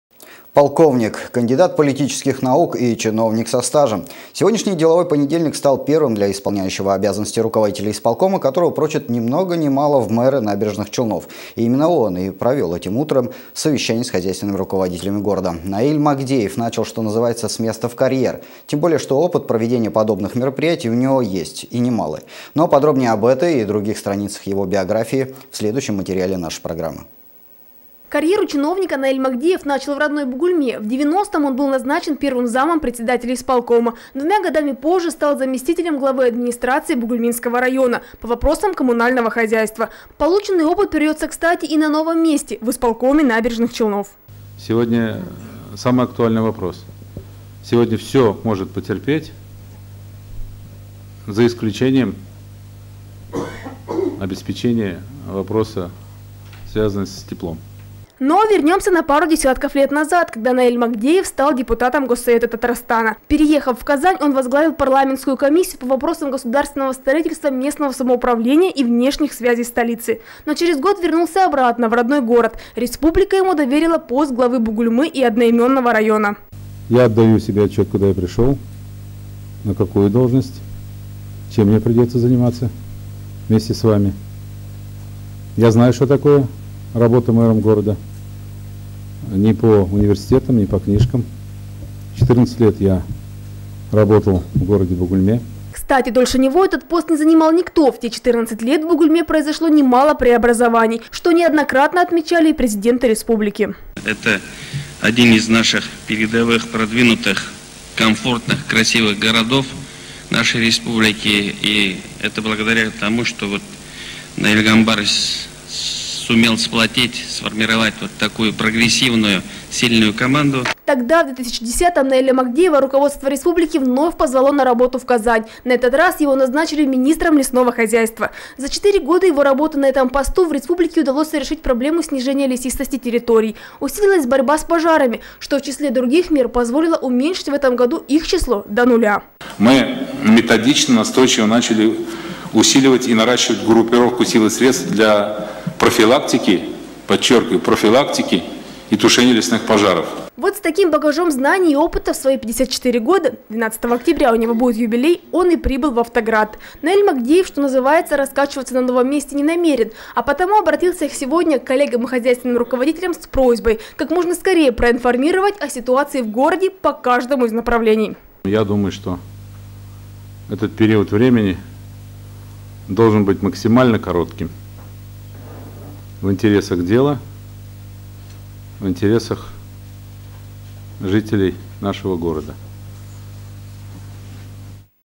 Кто он, Наиль Магдеев
Полковник, кандидат политических наук и чиновник со стажем. Сегодняшний деловой понедельник стал первым для и.о. руководителя Исполкома, которого прочат ни много ни мало в мэры Набережных Челнов. И именно он и провел этим утром совещание с хозяйственными руководителями города.